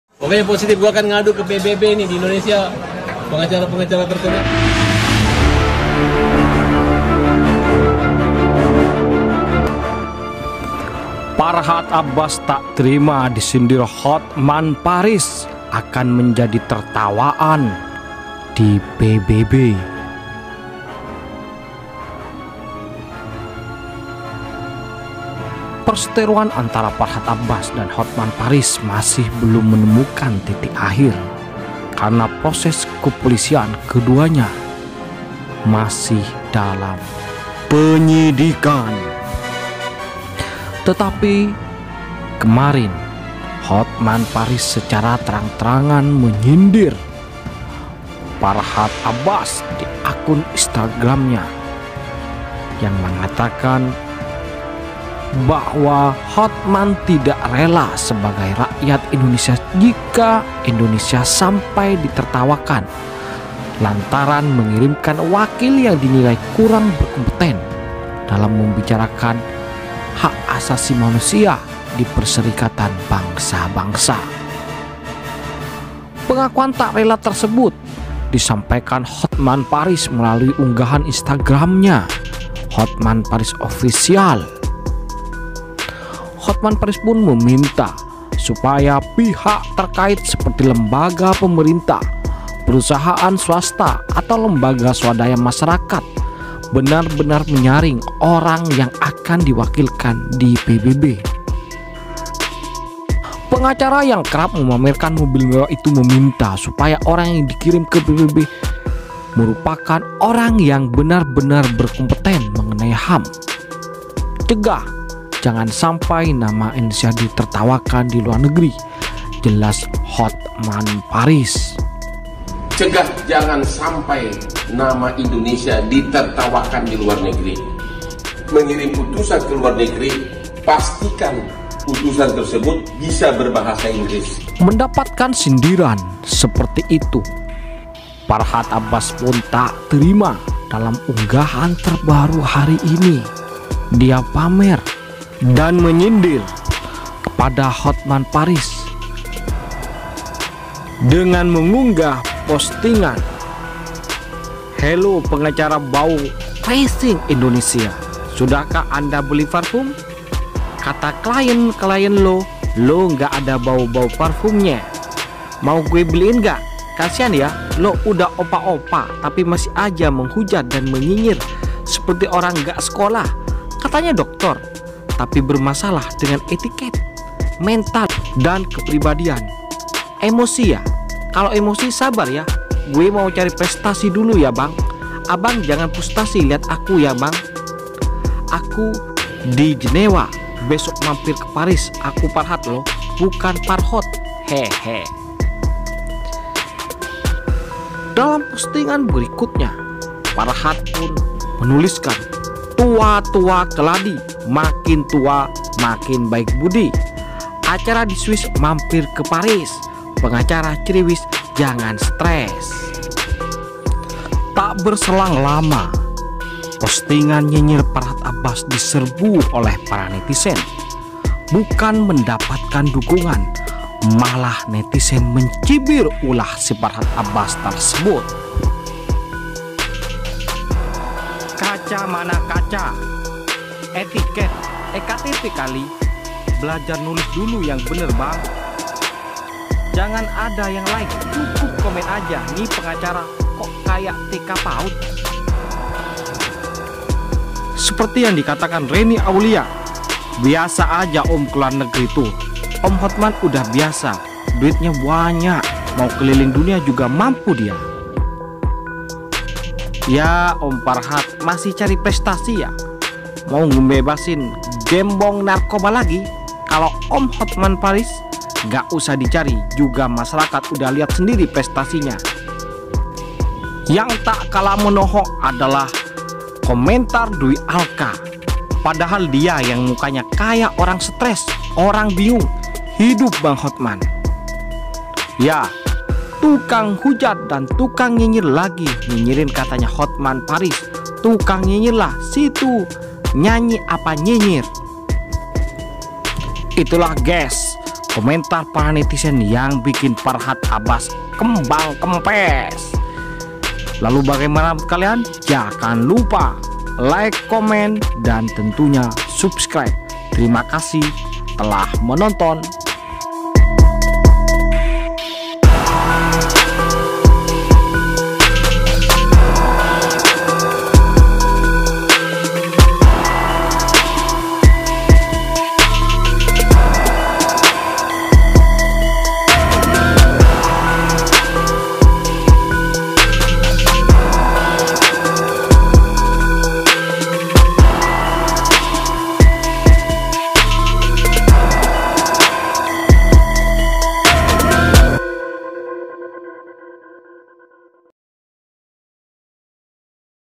Disindir Jadi Tertawaan PBB, Farhat Abbas Tak terima S€R4NGG Balik Hotman, Malah DinyinyiiiR Netizen
Disindir Hotman Jadi Tertawaan PBB, Farhat Abbas Tak terima S€R4NGG Balik Hotman Malah DinyinyiiiR Netizen

Jangan Lupa: _
Thankz!!..